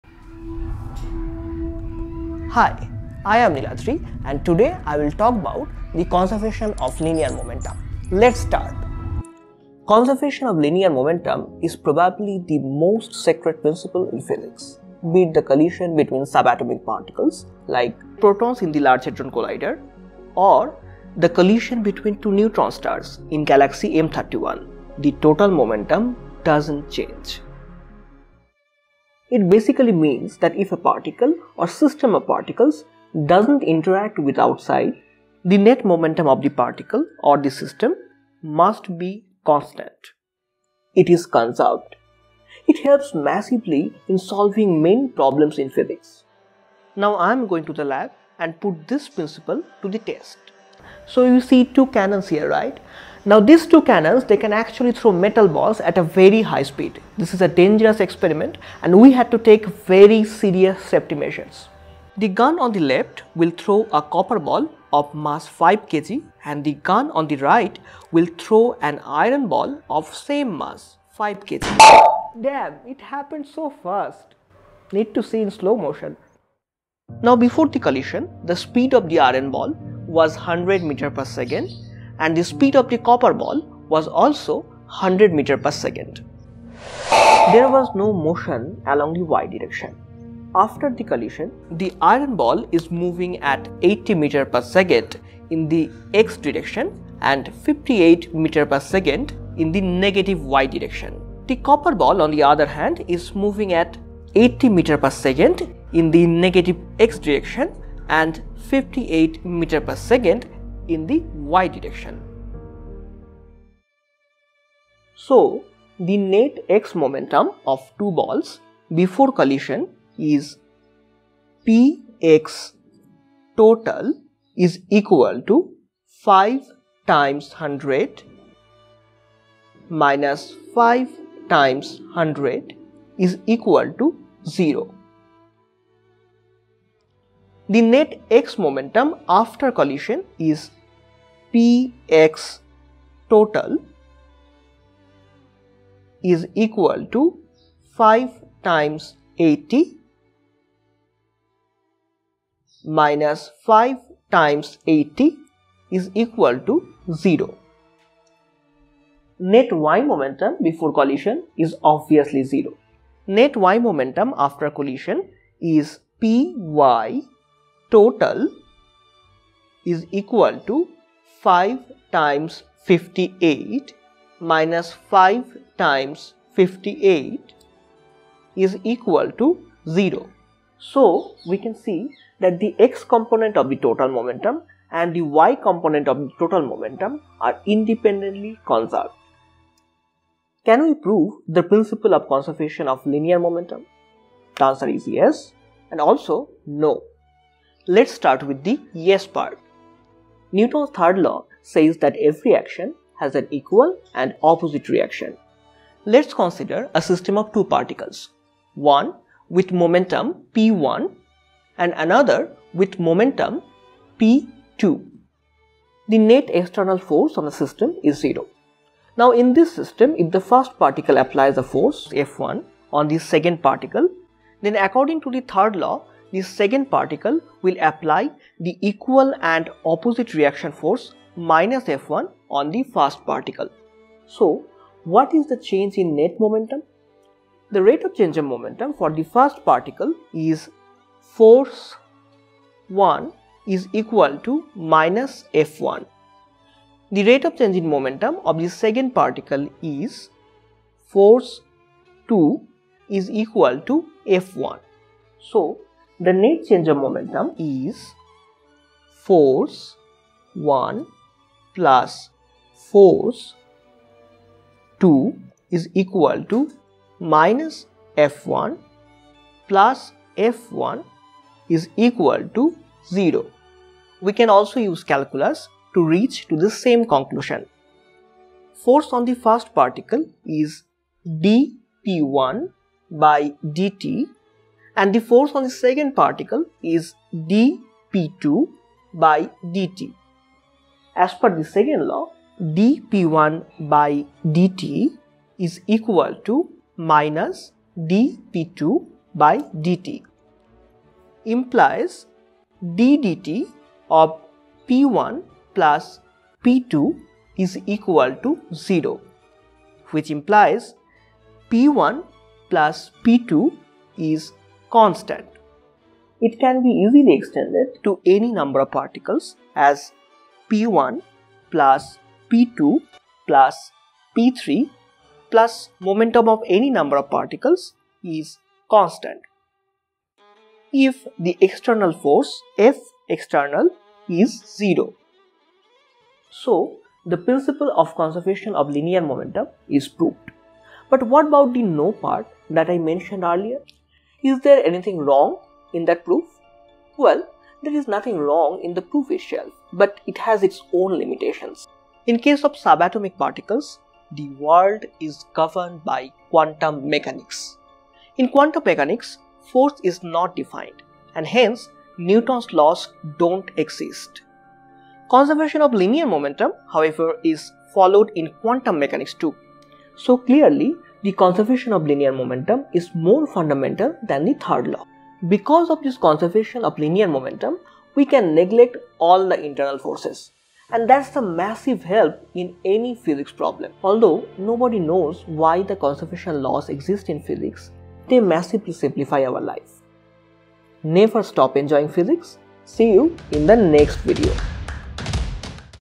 The most sacred principle in Physics: Conservation of Linear Momentum
Conservation of linear momentum is perhaps the most respected principle in Physics. Doesn't matter if you are dealing with subatomic particles of Quantum mechanics like proton, electron or something extremely large like Neutron stars or black holes, the law of conservation of momentum is followed.
It is very important to understand the law for anyone who is studying or interested in science. Whether you love physics or preparing for your class or the competitive exams like IIT-JEE, NEET, SAT or GRE, understanding the conservation of linear momentum is a must.